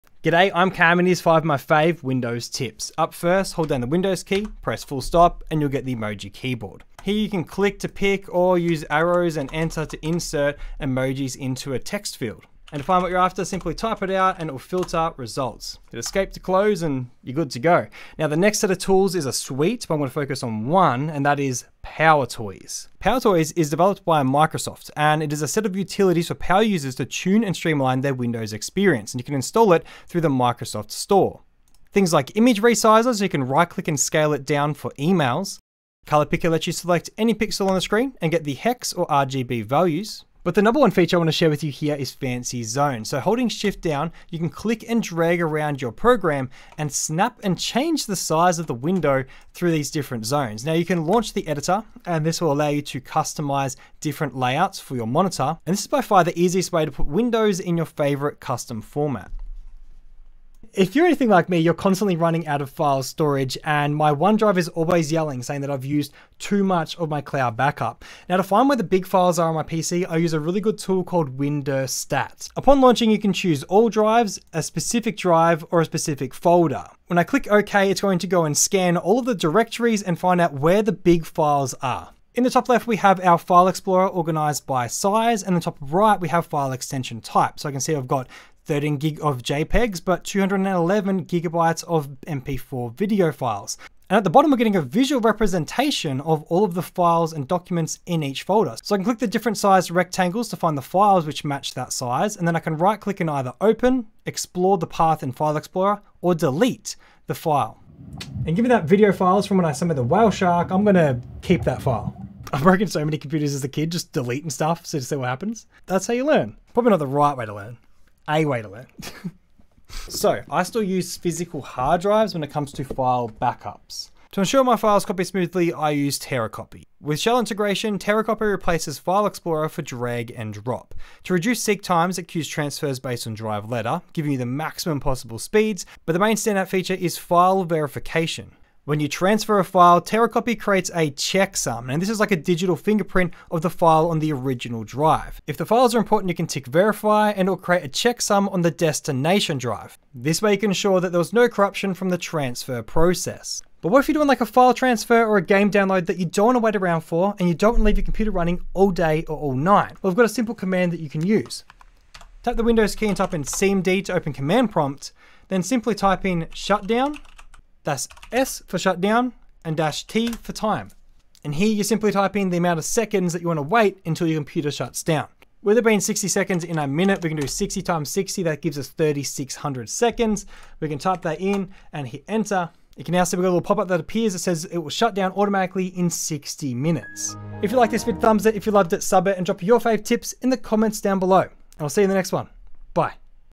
I wish I knew this years ago...
✅ LINKS
Emoji = [WIN]+[.]
Shutdown command prompt timer = shutdown -s -t 3600 (replace number with seconds)